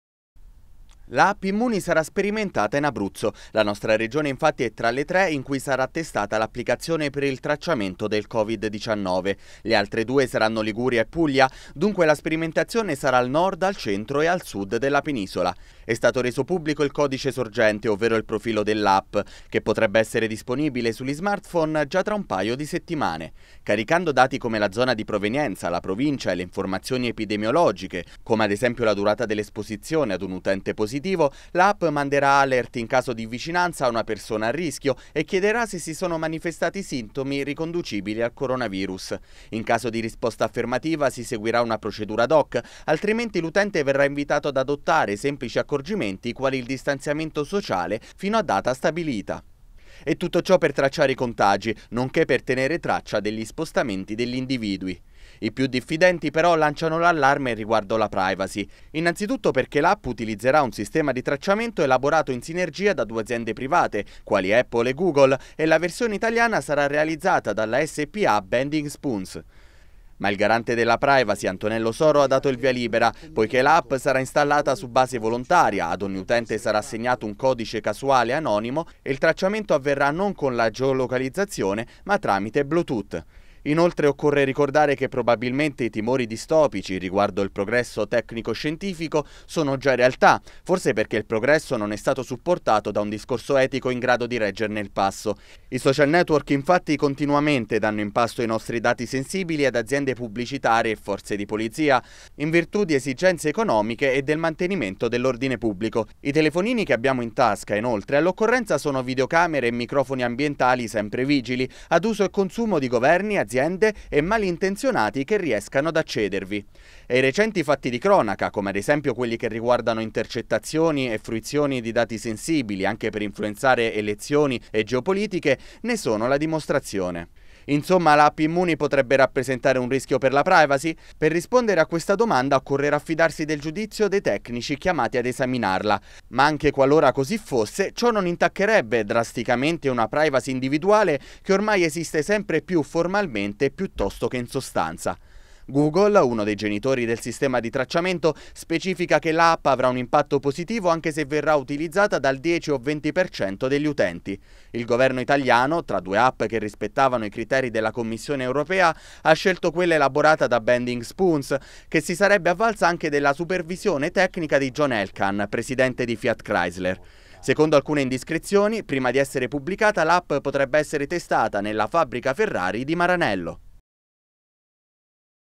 App Immuni. Al via la sperimentazione in Abruzzo
La app Immuni, l'applicazione per tracciare i contagi di Covid-19, sarà sperimentata in Liguria, Abruzzo e Puglia e potrebbe essere scaricabile già tra un paio di settimane. Perplessità riguardo la privacy, ma il garante Soro ha dato il via libera.